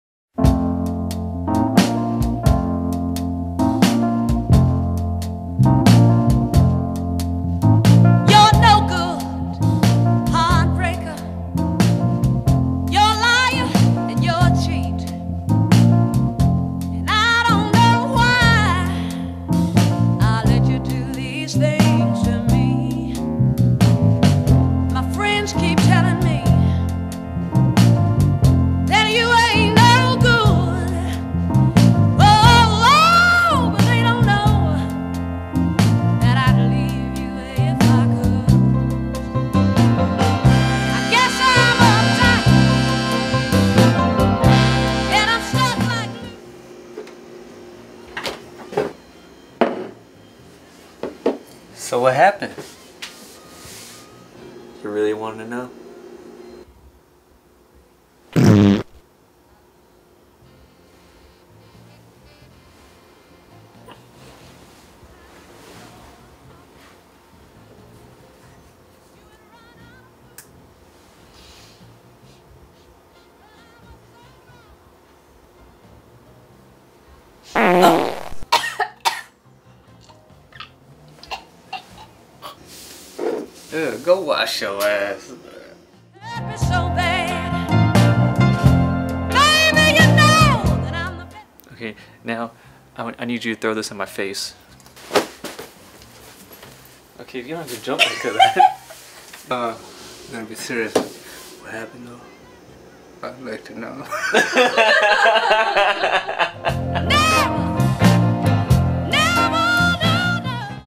Stank
Cuzin Sean falls victim to a natural cause that he has no control of and he lady kicks him out. Enjoy!